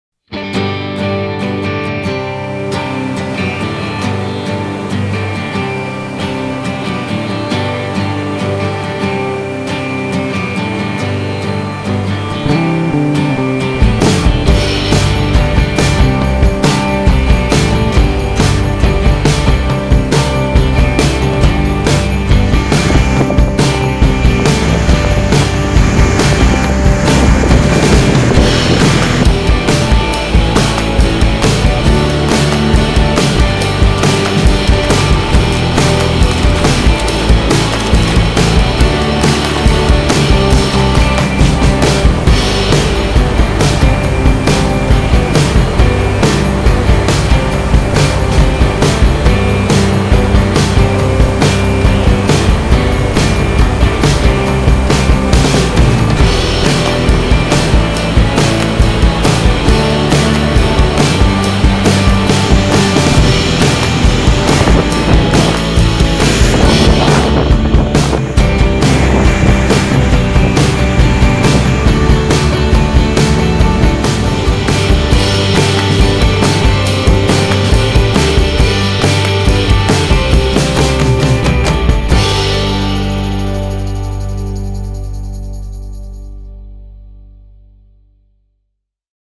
Cachoeira do Prata Juscimeira MT
Cachoeira do Prata fica em Juscimeira localiza-se há 160 km de Cuiabá, sobre o  Rio Prata.